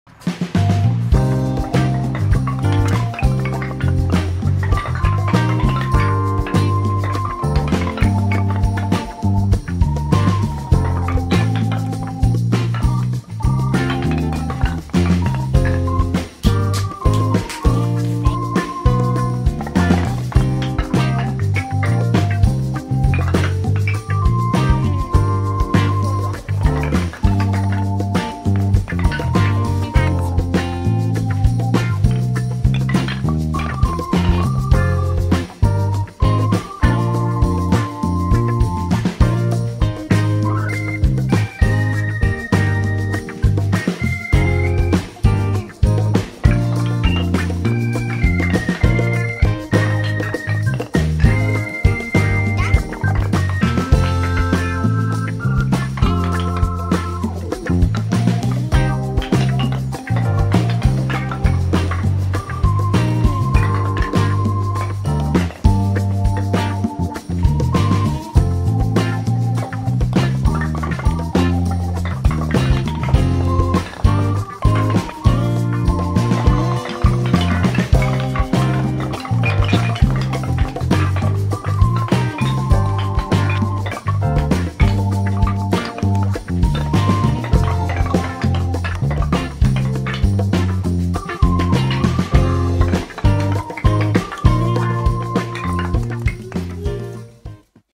Ball Track in a Barn Sound of Wood Ball Rolling Down the Wood Track
All this wood is collected for long cold months ahead. It's kept in a barn that is not used otherwise any longer. Farming life has ended.
We found some cut off bits and made a ball track on the huge pile of chopped (with and axe) wood. Here is out track!
Hope you enjoy the video and the sound of wooden balls rolling down the wooden track.